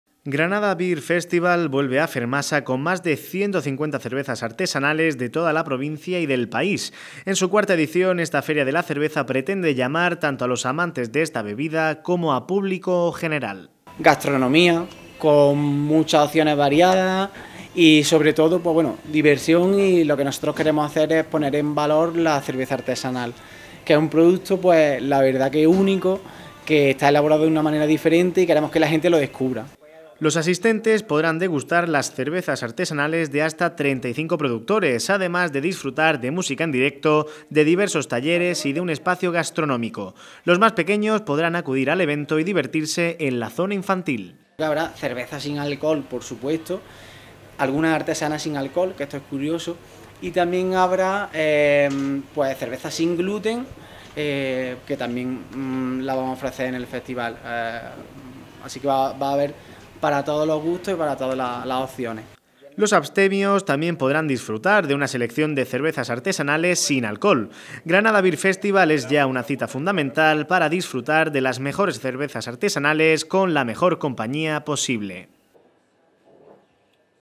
Granada Beer Festival el 5 y 6 de Abril en Granada
La cuarta edición de Granada Beer Festival llegará a la Feria de Muestras (FERMASA) de Armilla en el primer fin de semana del mes de abril. Todos sus asistentes podrán degustar la cantidad de 150 cervezas artesanales durante los días 5 y 6 de abril. Además de la cerveza artesanal de hasta 35 productores de la provincia y del resto de España, los asistentes podrán disfrutar de cervezas sin gluten o de cervezas sin alcohol, un espacio gastronómico apto también para vegetarianos, música en directo de bandas como Mente Fría o D'Baldomeros y de diversos talleres y catas. Los más pequeños podrán divertirse en el área infantil y no perderse esta cita fundamental para los amantes y estudiosos de la cerveza.